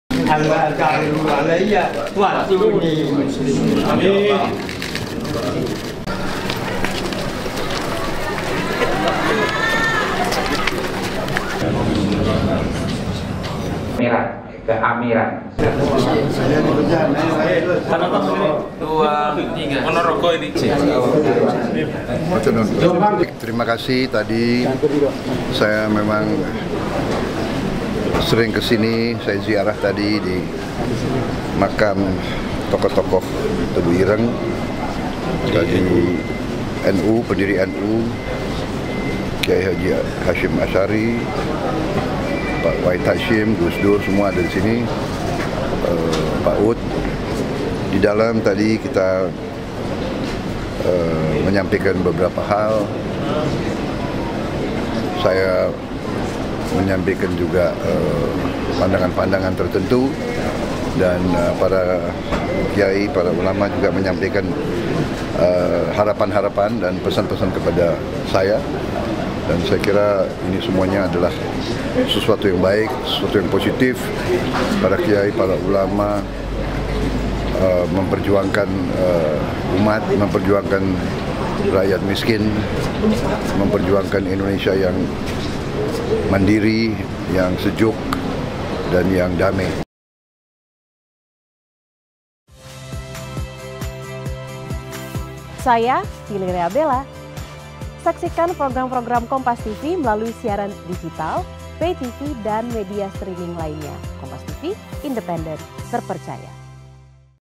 Momen Prabowo Subianto Temui Sejumlah Kiai Saat Berkunjung ke Ponpes Tebuireng di Jombang
Jombang, Kompas.TV Jawa Timur -  Bakal Calon Presiden (Bacapres) Prabowo Subianto berkunjung ke Pesantren Tebuireng, Jombang pada Sabtu (11/11/2023)

Dalam kesempatan itu, Prabowo sempat ziarah ke makam pendiri NU KH Hasyim Asyari, Ayah Gus Dur KH Hasyim Wahid serta makam Gus Dur dan Gus Sholah.

Prabowo juga melakukan pertemuan dengan puluhan kiai dan pengasuh pesantren dari Jombang dan Mojokerto.

Selain meminta restu, pertemuan ini bertujuan untuk merekatkan hubungan serta menggali aspirasi.